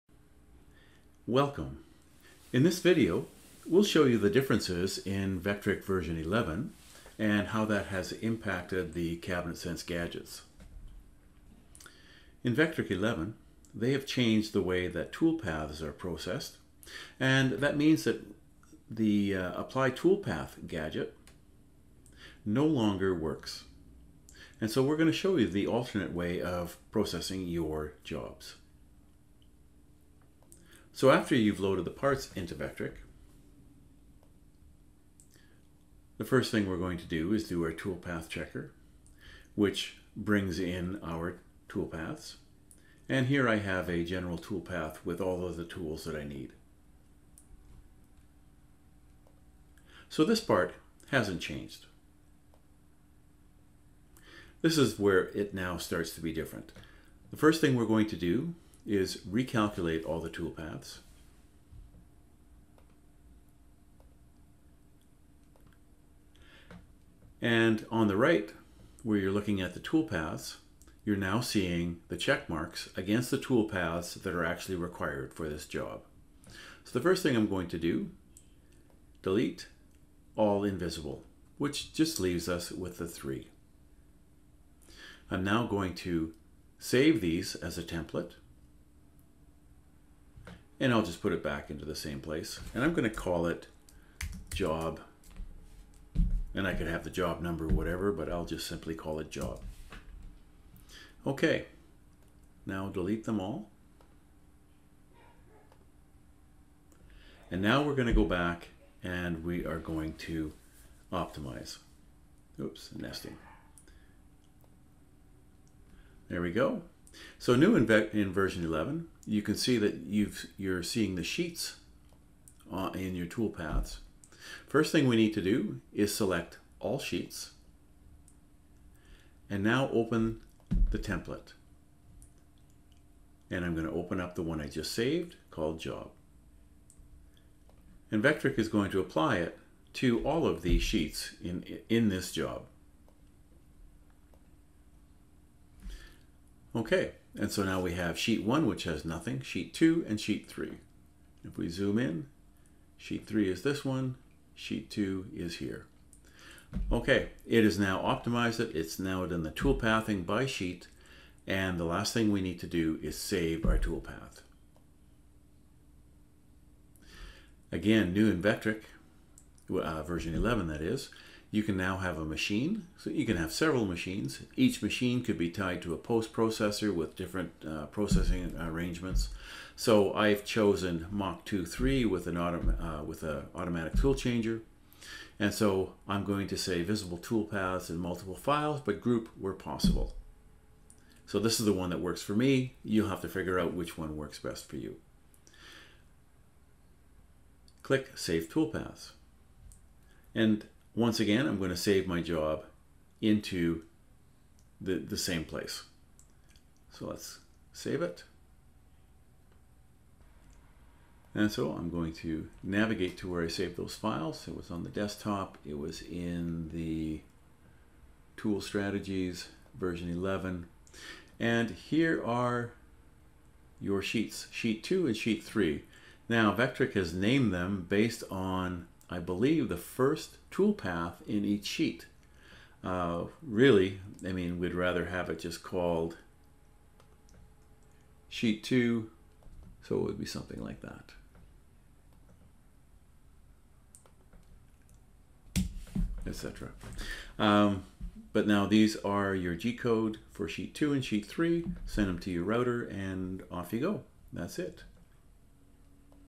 Vectric V11.0 Changes
V11.0 of Vectric has changed the way that CabinetSense jobs are processed.  This video will show you what has changed and the new flow to be followed for your jobs.